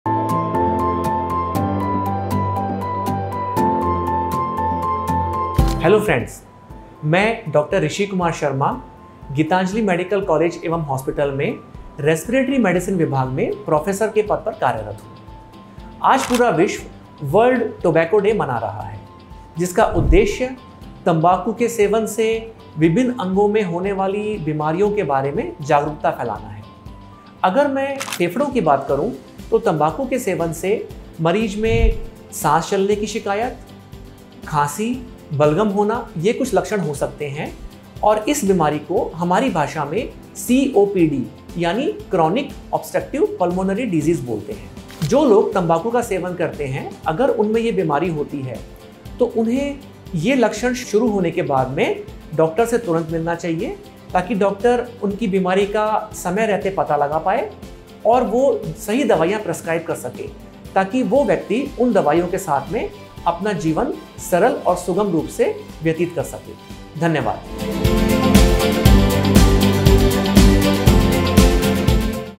Rishi Kumar Sharma: World No Tobaaco Day
हर साल 31 मई को 'विश्व तंबाकू निषेध दिवस' (World No Tobacco Day) मनाया जाता है। जिसका उद्देश्य लोगों को तंबाकू से होने वाले खतरों के बारे में बताना और जागरूक करना है। आइये जानते है इस वीडियो के माध्यम से ।